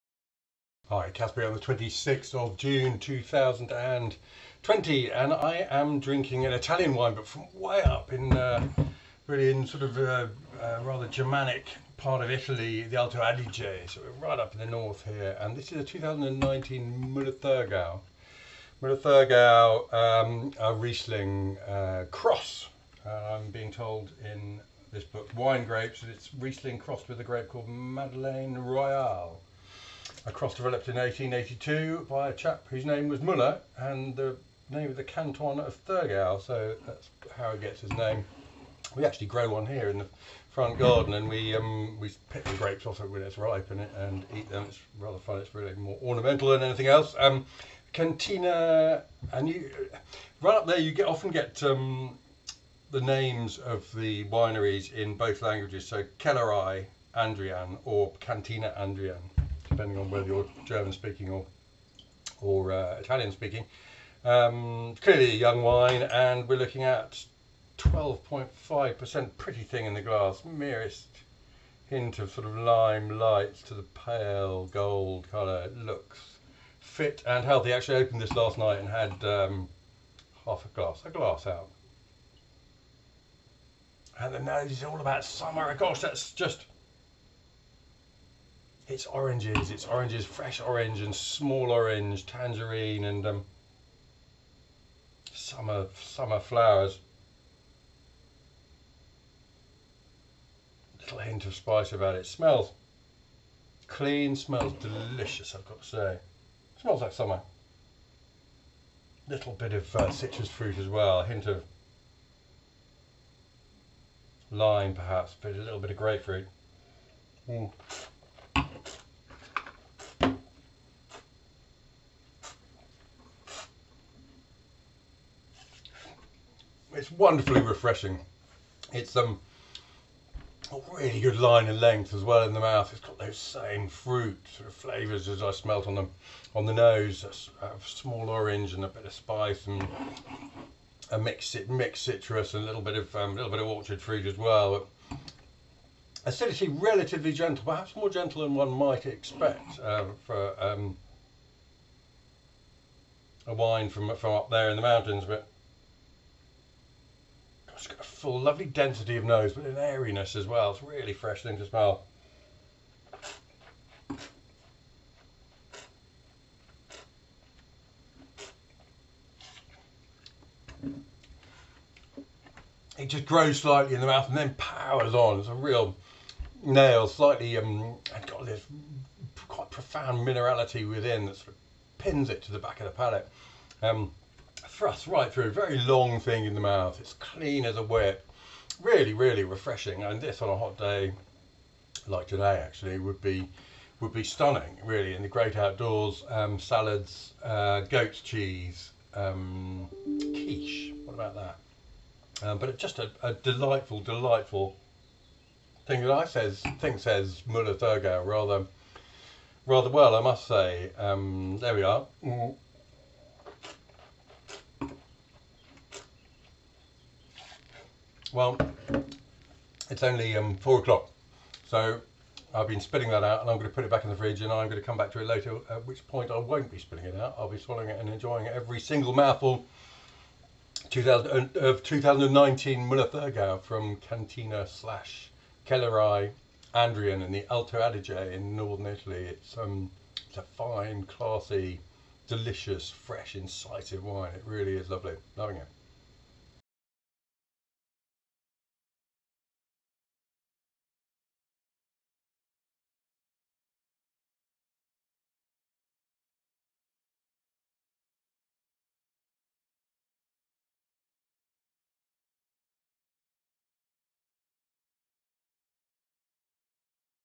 2019 Muller Thurgau, Cantina Andrian - 26th June 2020
Caspar Bowes tastes a dry white wine from the far north of Italy and finds a wonderfully clean, varietal expression